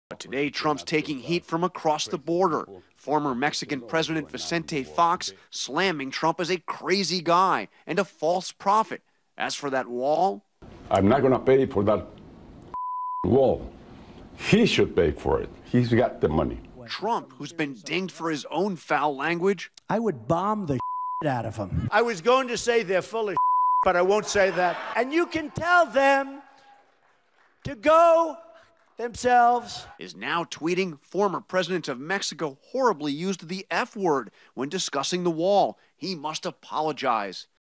Ex-Mexican President To Trump 'Not Going To Pay For That F Wall'
February 26, 2016 - Former Mexican president Vicente Fox had strong -– albeit naughty -– words for Donald Trump’s controversial plan to force the Mexican government into paying for a border wall.

“I am not going to pay for that f wall,” he told Fusion's Jorge Ramos. “He should pay for it. He’s got the money.”